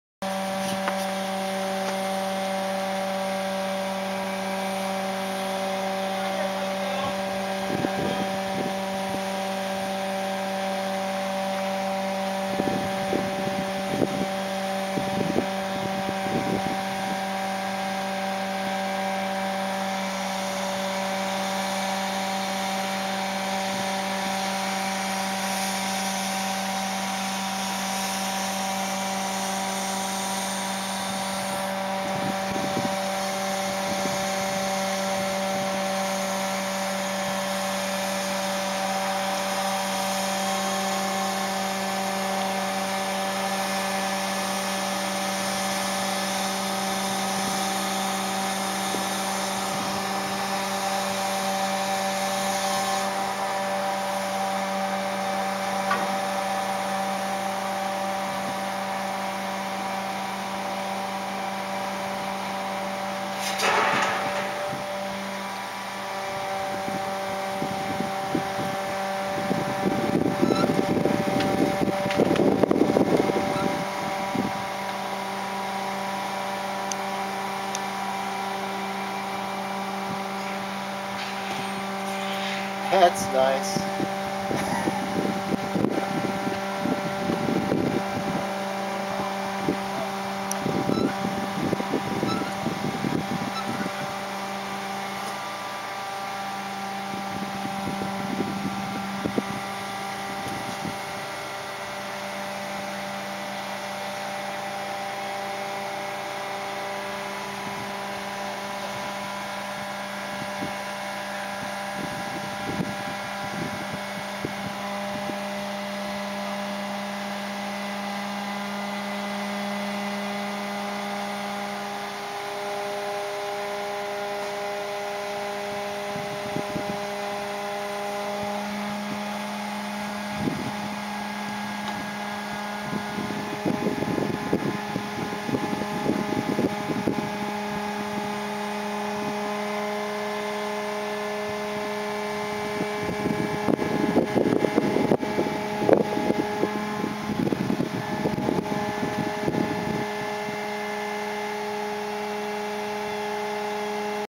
raising_tower.wmv
Raising the wind tower at the farm of Suzanna Raker and Fred Galloway near Calumet, Michigan in May 2011.